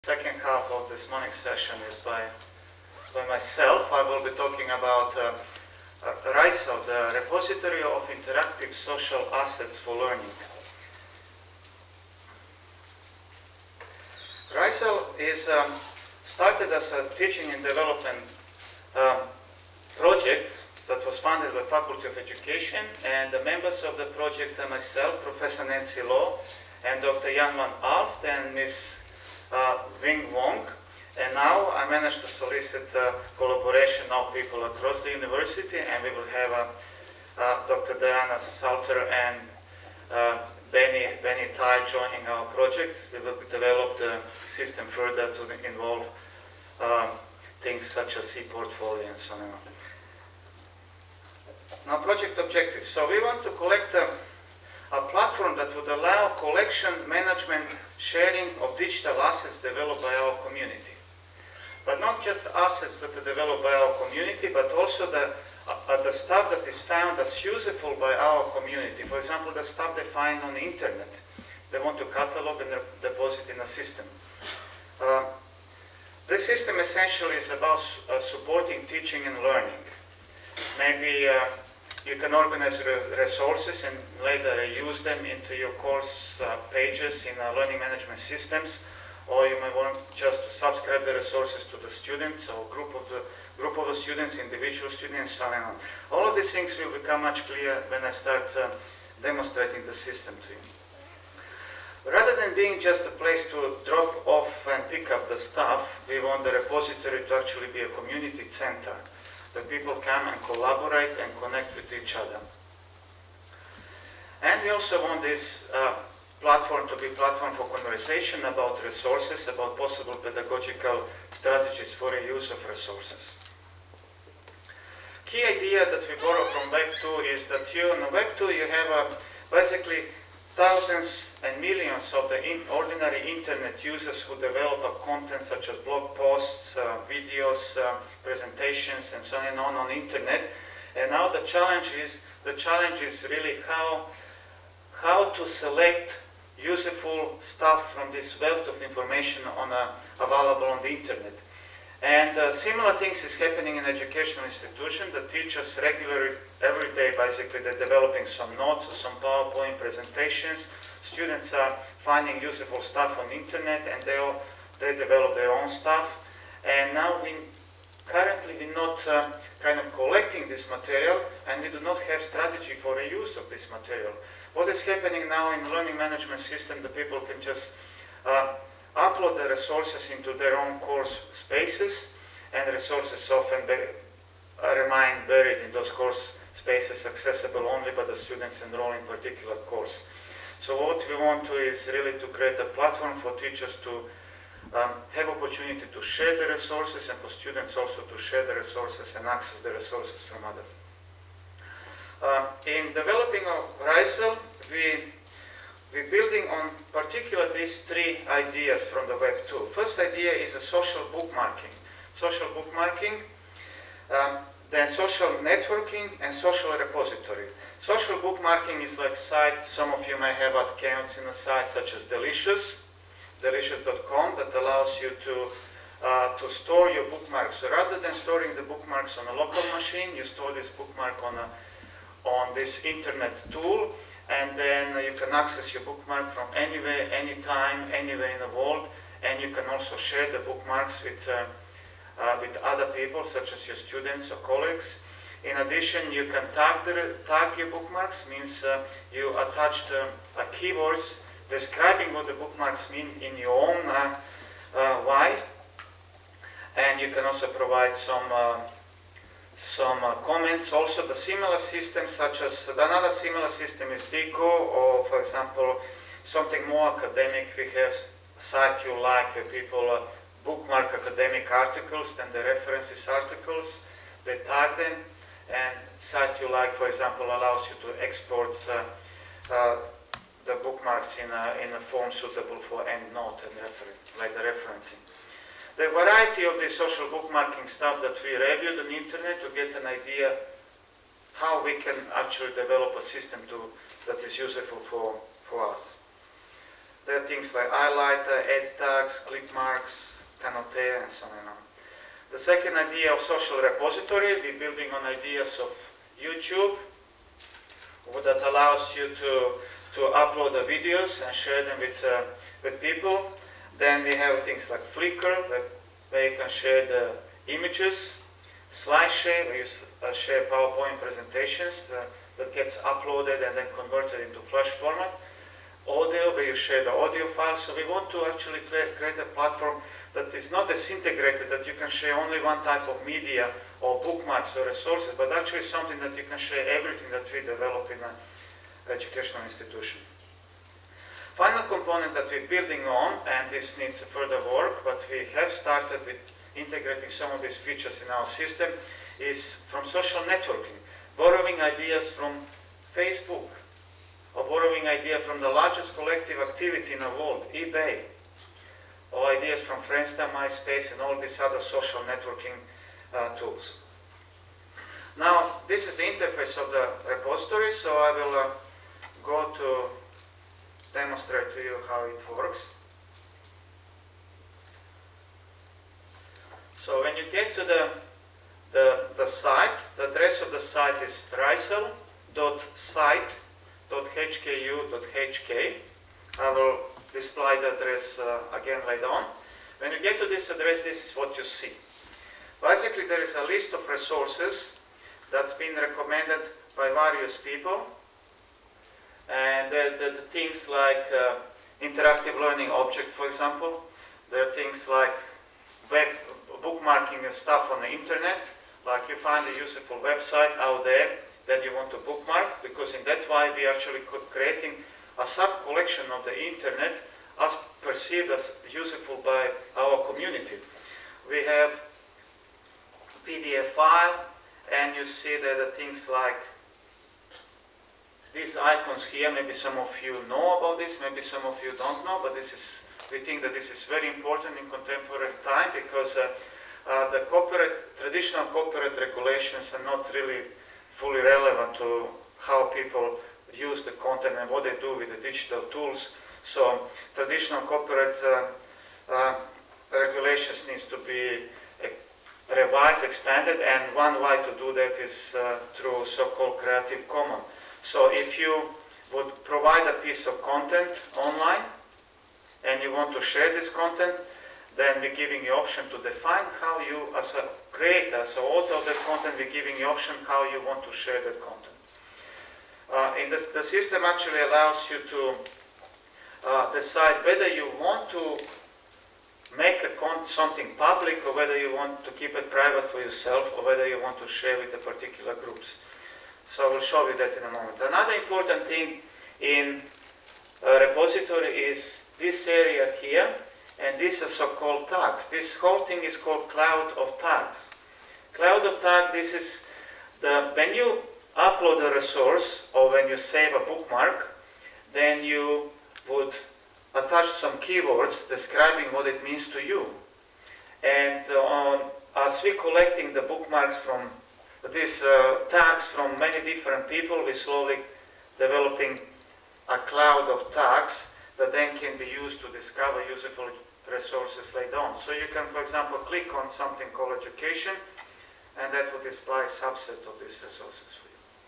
RISAL@HKU - Dr. Daniel CHURCHILL (Part 1)
RISAL@HKU - Bookmarking, Repository, Networking and Portfolio for Learning in the Age of the Web 2.0 | Date:  6 March 2009 (Friday) | Speaker: Dr. Daniel CHURCHILL, Assistant Professor, Faculty of Education, The University of Hong Kong